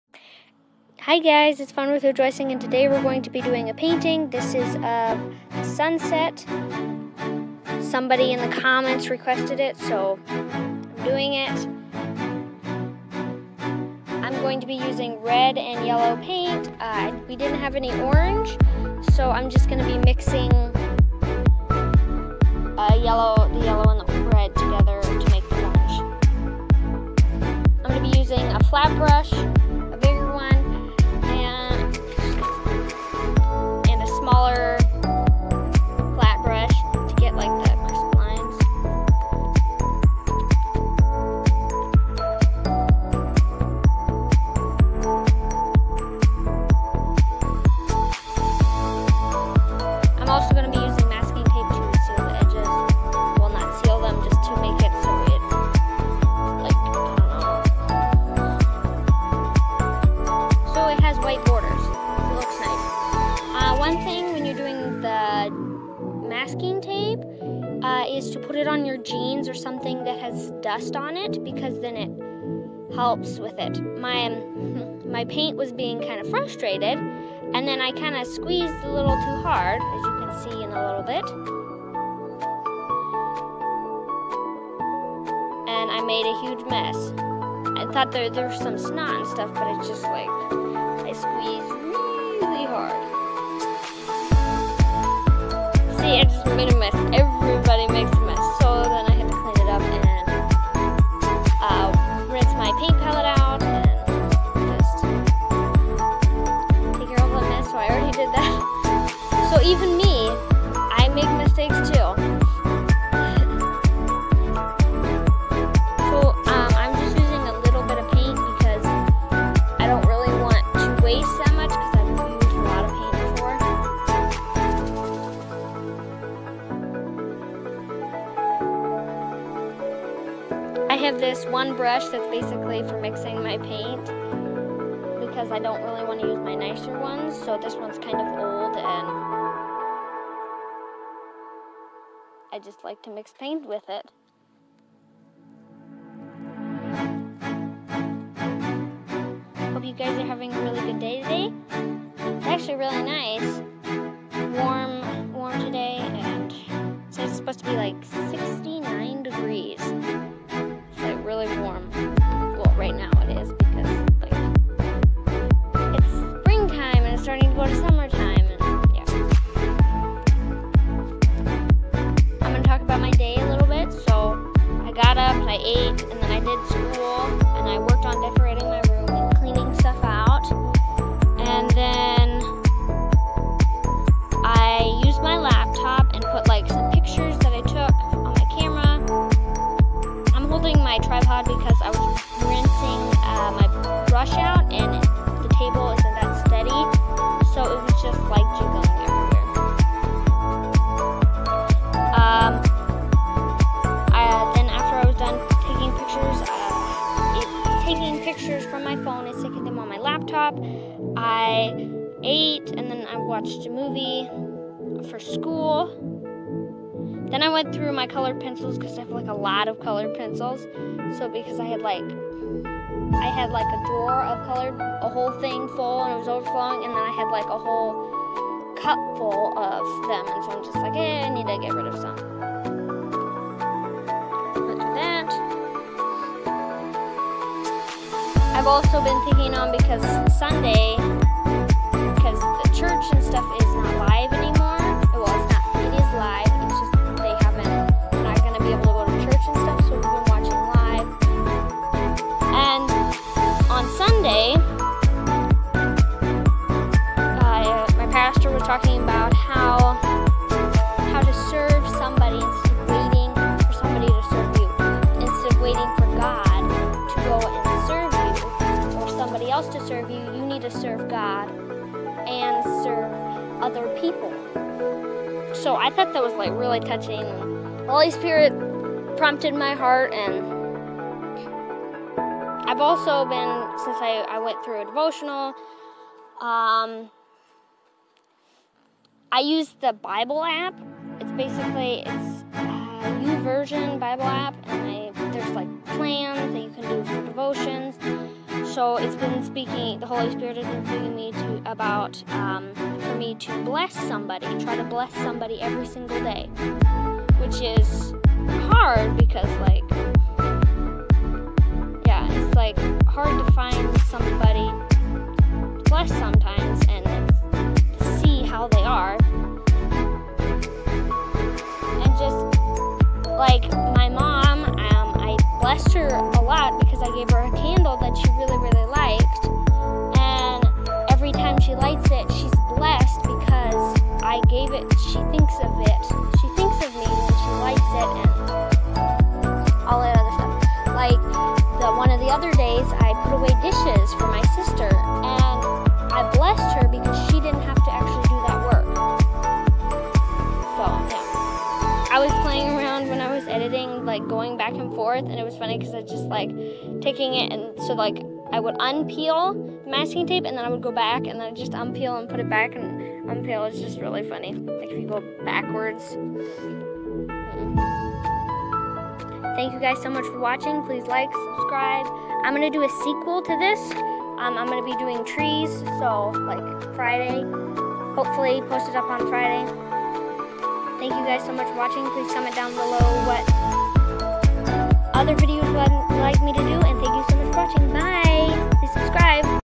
How to paint a sunset.
Hey guys! Today I'm painting the sunset. I do bullet journal, and painting video. I'm also planning on doing more videos in the future. I hope you really liked it. Give a big thumbs up 👍

Here is the link to my church.

Here is the bible app I use.

Paint. 20414 real yellow. 20403E white. 20415E real red.
Masking tape.
Paint brushes. 2 flat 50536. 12 flat 50536.
Painting pallet.

You cut - video editer.
Voiceover

Musician: @iksonofficial